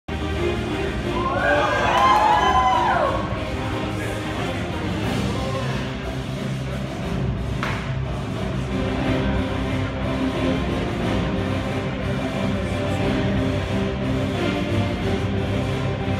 CrossFit Insanity Erena on the Cyr Part 1
CrossFit Insanity 2015 Holiday Party and 5 Year Anniversary, Client Erena on the Cyr.